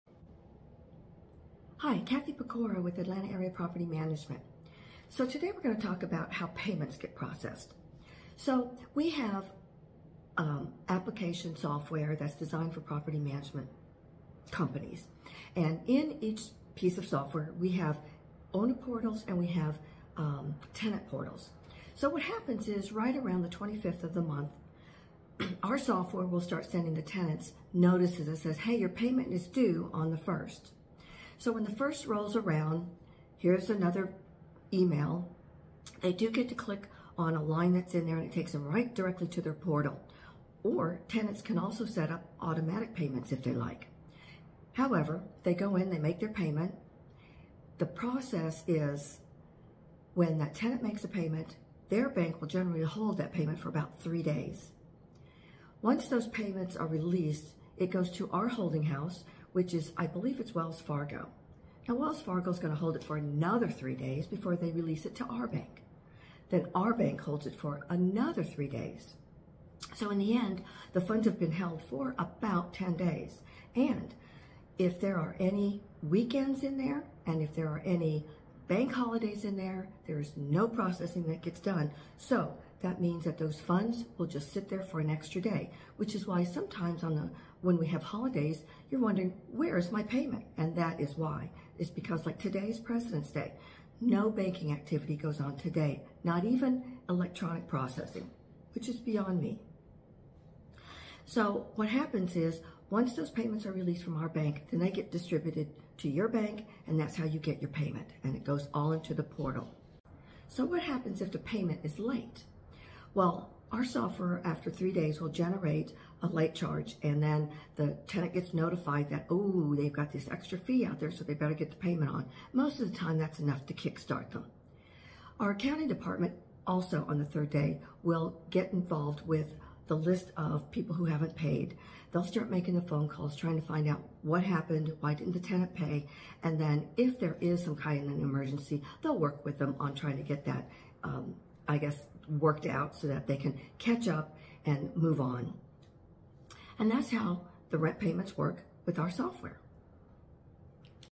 Tenant Payments
We use a property management software system to process tenant payments.